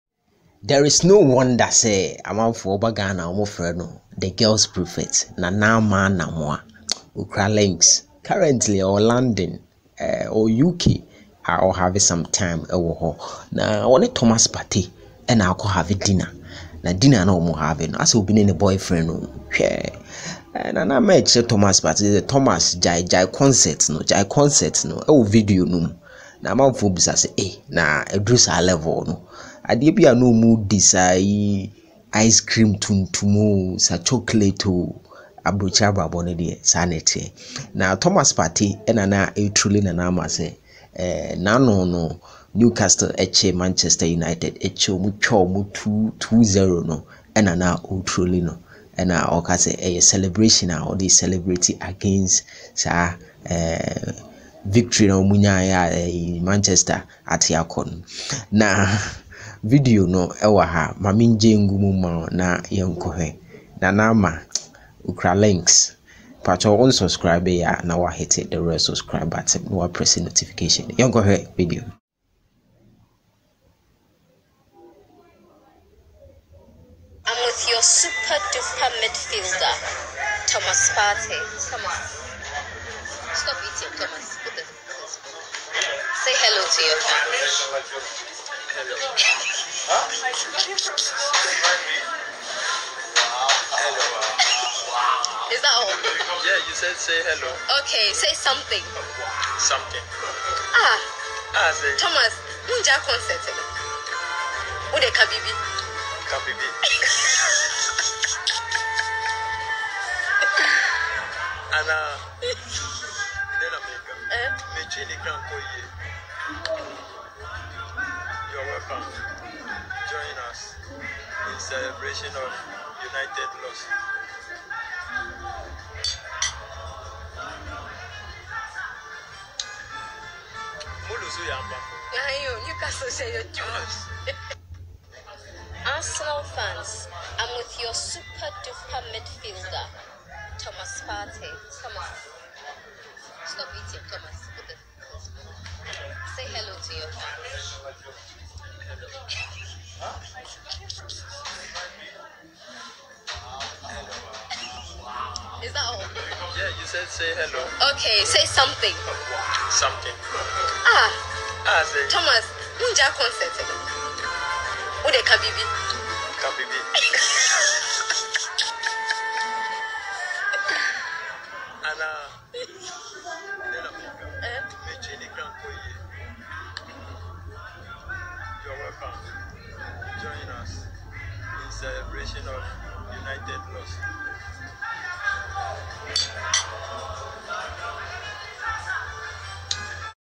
Asε Obi Ne Ne Girl: "Thomas Gyae Concert No" Nana Aba Tells Partey As Both Chill In London
A video of Thomas Partey and Nana Aba Anamoah Chilling in London.